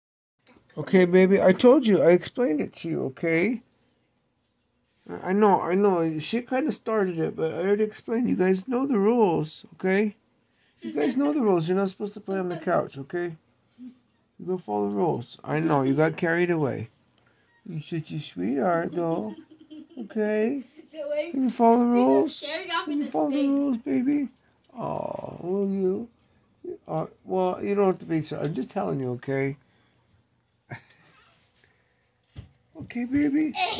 DON'T GIVE ME THAT LOOK pup..
cute but sad pup didn't want to be talked to about playing on the couch